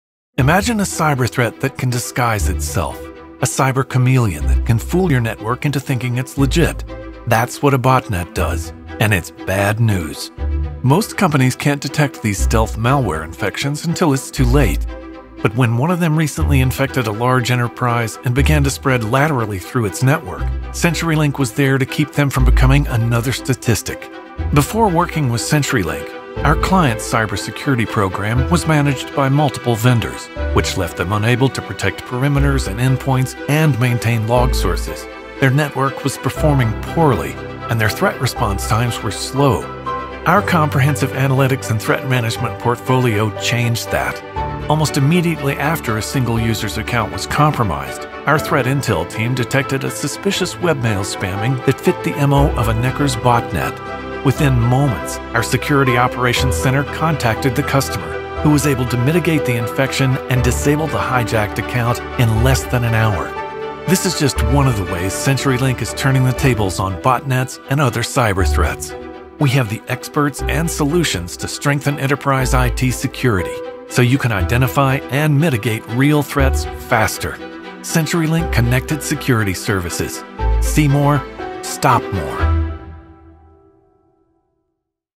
CenturyLink Connected Security Services. See More. Stop More.
At CenturyLink, our security builds on two fundamental directives: to leverage our expansive  global threat visibility and to act against the threats we see. CenturyLink is serious about doing its part to help protect the internet, including disrupting the work of bad actors and providing insights and recommendations for enterprise defenders to safeguard their networks.

CenturyLink offers security log monitoring, adaptive threat intelligence, and incident management and response services.